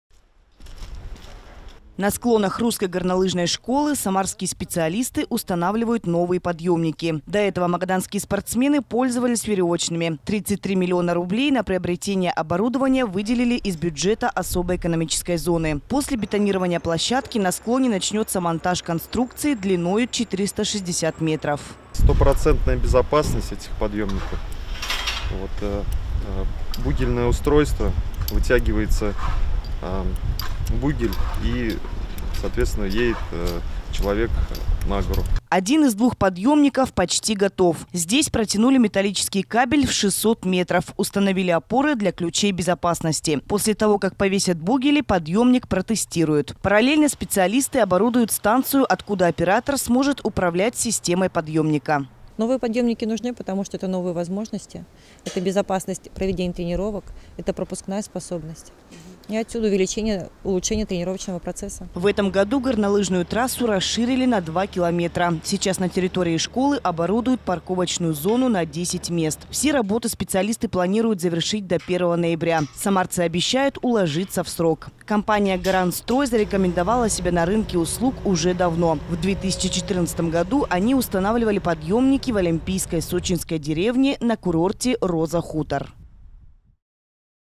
мтквидео Новые подъемники на склонах русской горнолыжной школы
На склонах русской горнолыжной школы самарские специалисты устанавливают новые подъёмники. До этого магаданские спортсмены пользовались веревочными. 33 миллиона рублей на приобретение оборудования выделили из бюджета особой экономической зоны. После бетонирования площадки, на склоне начнется монтаж конструкции длиною 460 метров.